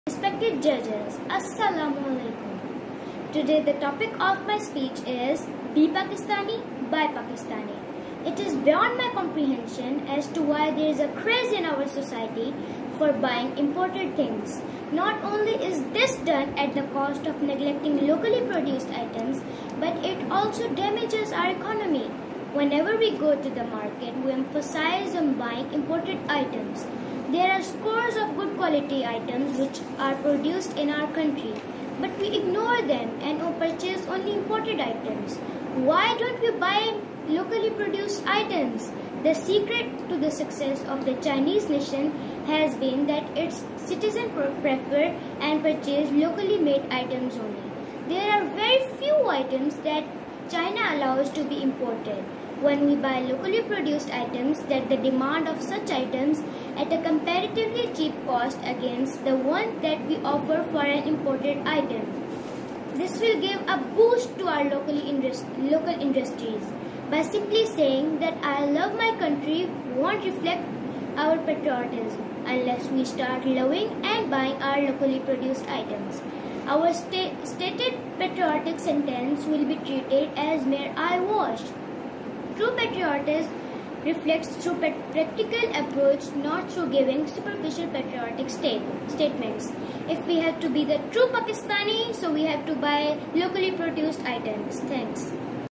Azadi Competition -2020 in Two Categories
Name of Participant: Azka Qadeer, Institute: The City School Parco Chapter Qasba, Category: Speech, City: Gujarat